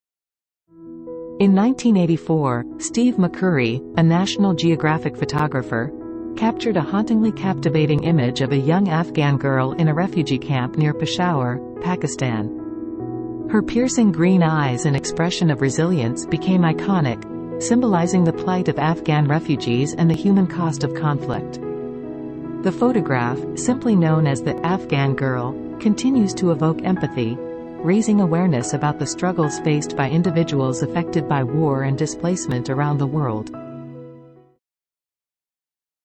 Afghan Girl #10
In 1984, Steve McCurry, a National Geographic photographer, captured a hauntingly captivating image of a young Afghan girl in a refugee camp near Peshawar, Pakistan. Her piercing green eyes and expression of resilience became iconic, symbolizing the plight of Afghan refugees and the human cost of conflict. The photograph, simply known as the "Afghan Girl," continues to evoke empathy, raising awareness about the struggles faced by individuals affected by war and displacement around the world.